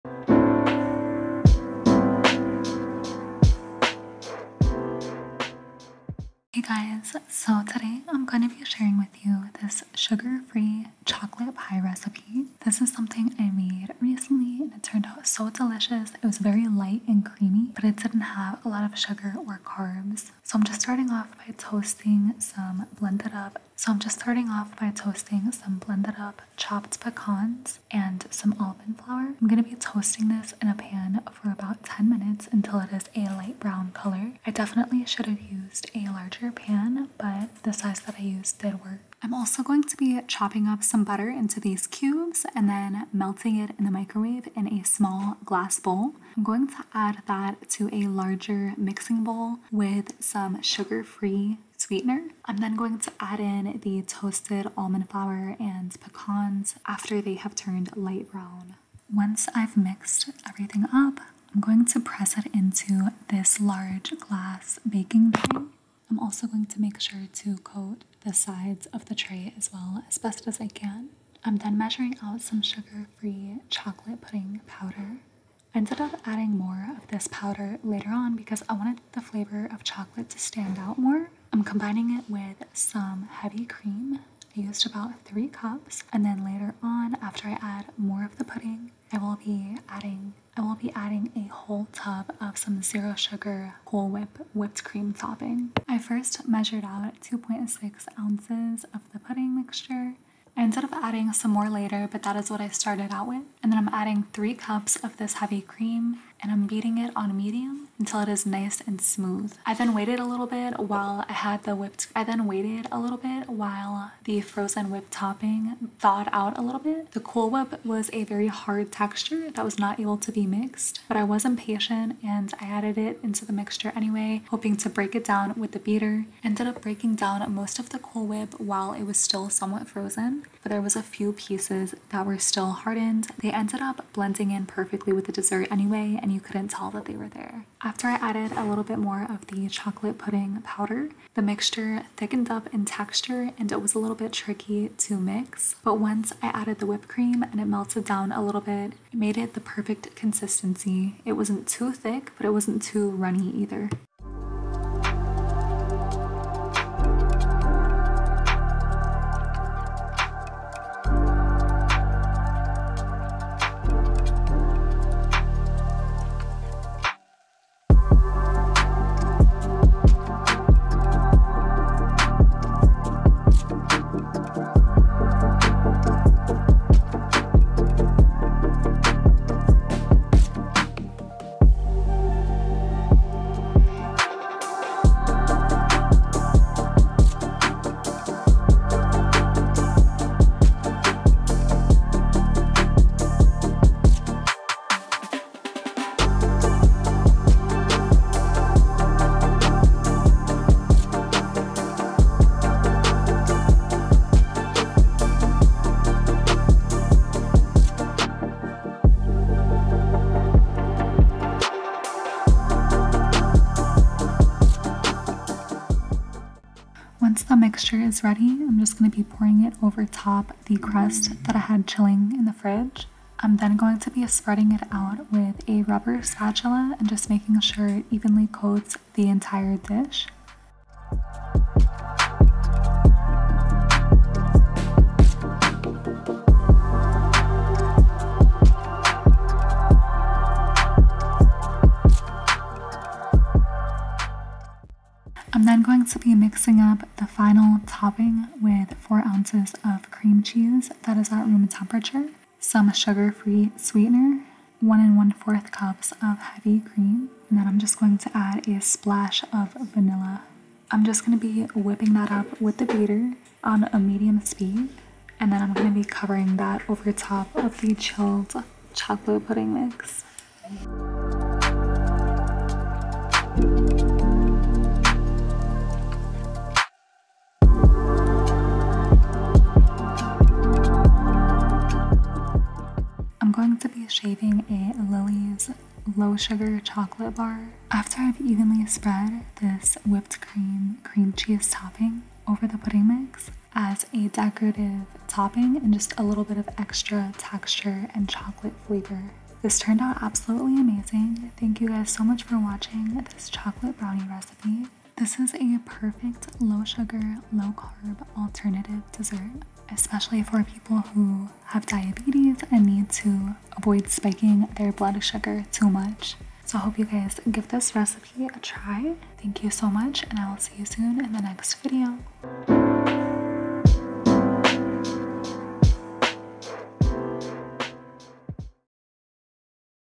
Sugar free diabetic friendly chocolate pie recipe!
This low sugar low carb chocolate pudding pie recipe is so delicious, creamy and rich. Everybody loved this chocolate pie and the filling and topping paired so well with the toasted pecan crust. The recipe is linked below, feel free to try this delicious dessert perfect for diabetics or those limiting blood sugar spikes!